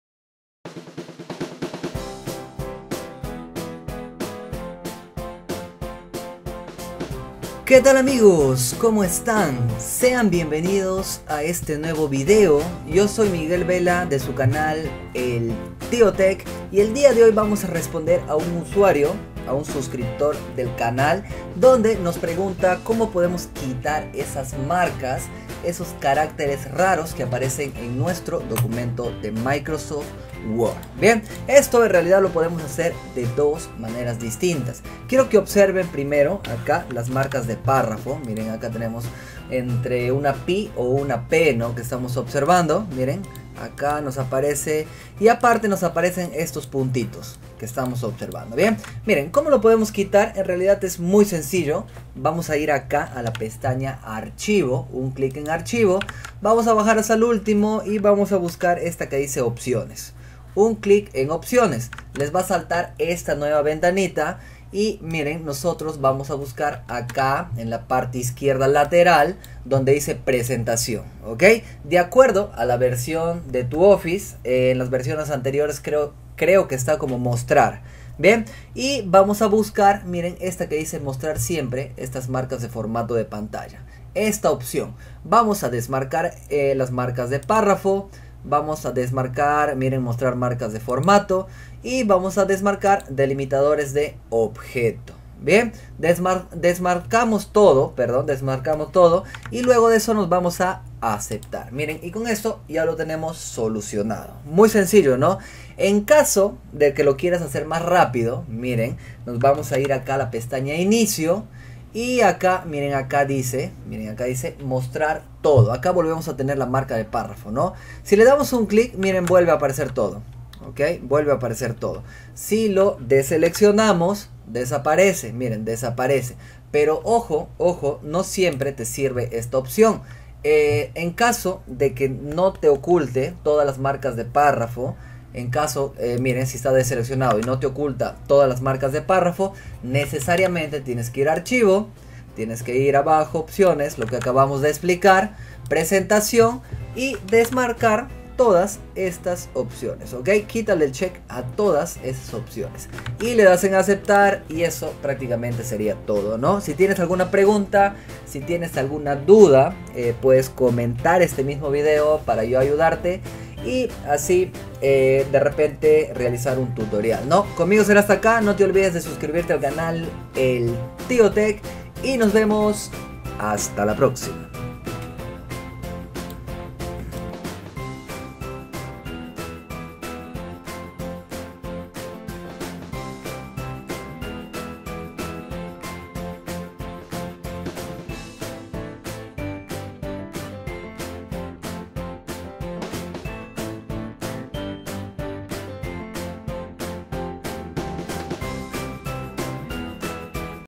Como quitar marcas de párrafo en WORD (Eliminar símbolos raros Word)
Aprende a como quitar marcas de párrafos en WORD, eliminar esos símbolos raros que aparecen de la nada en WORD.